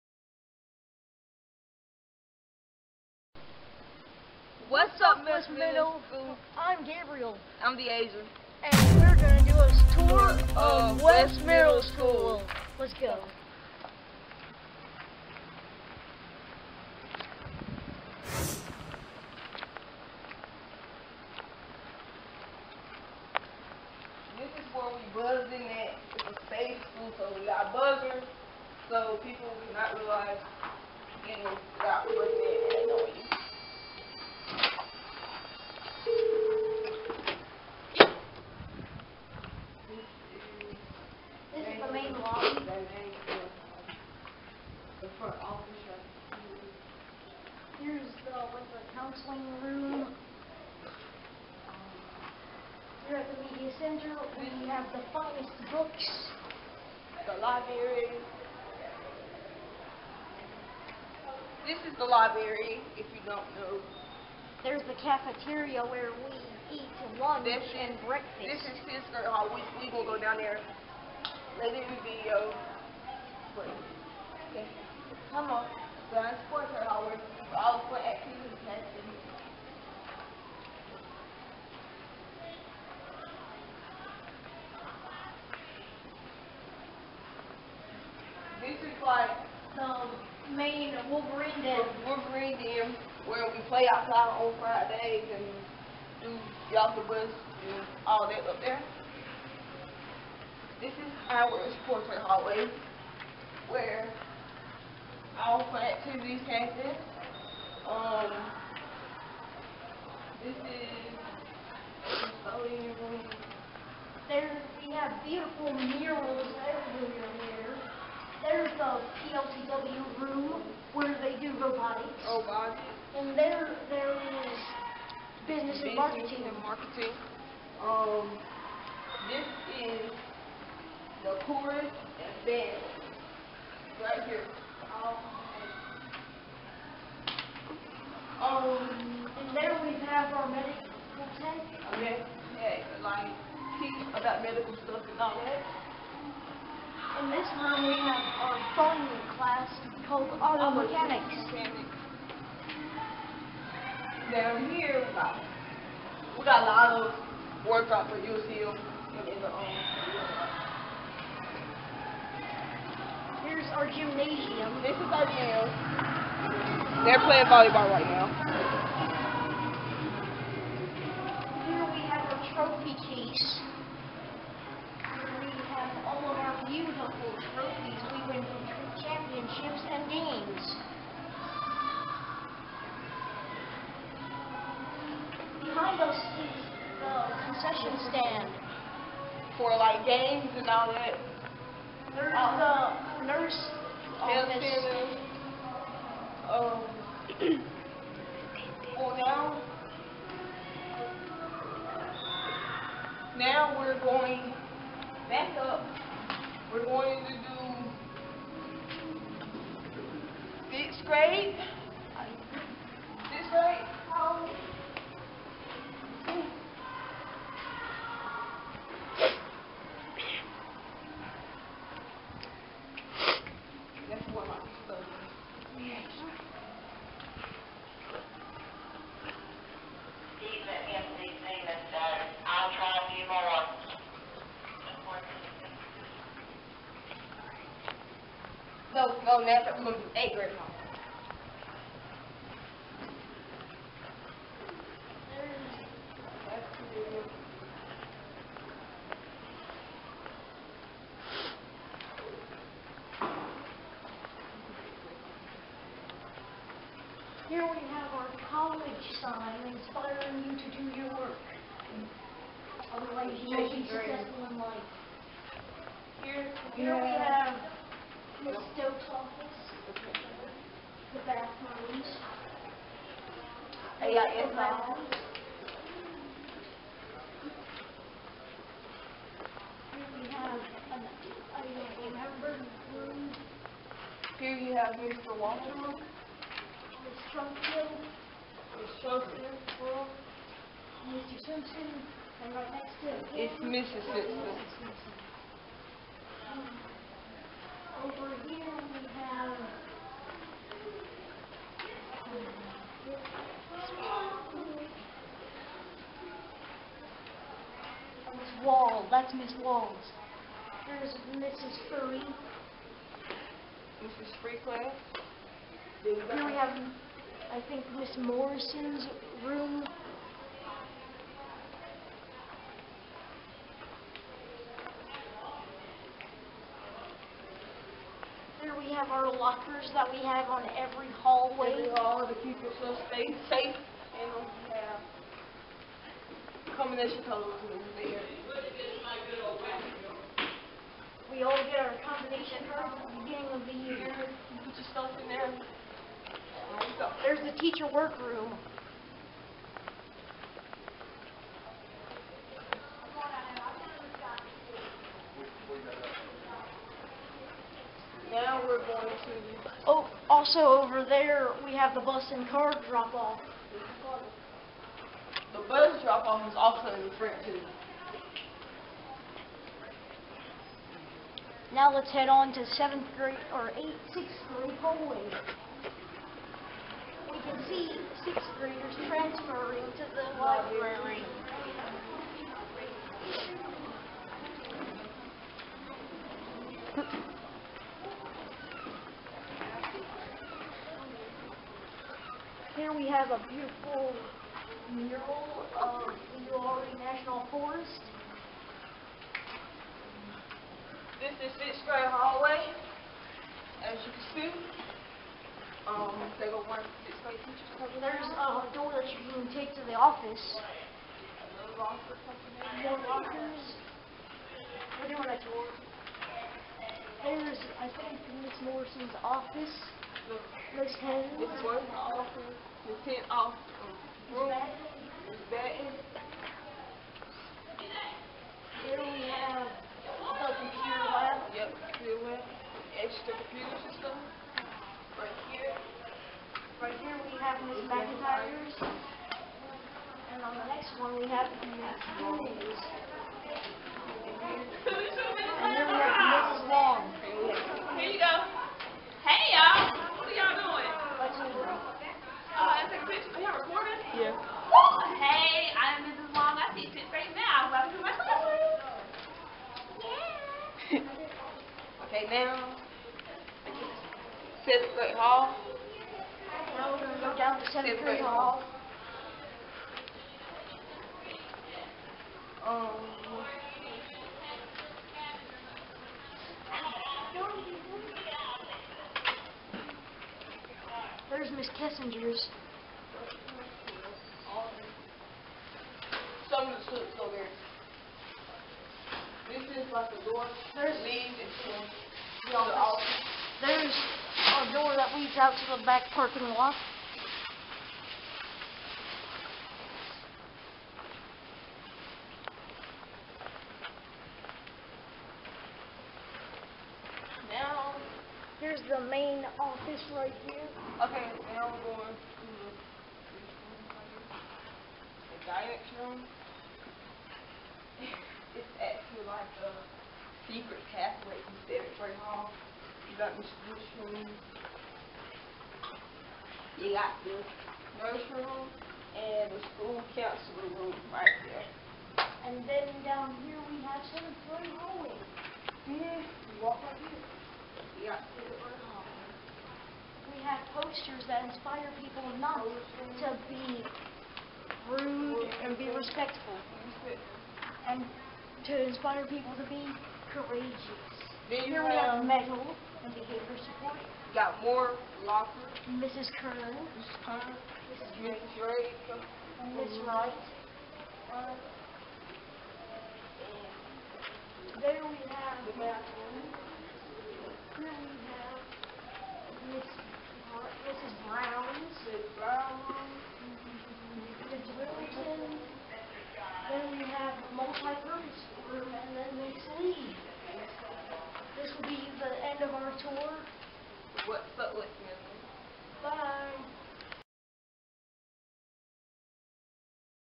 What's Up West Middle! Episode 2 "A tour of the school!"
In this episode Gabe and D'aja give you a personal tour of the school.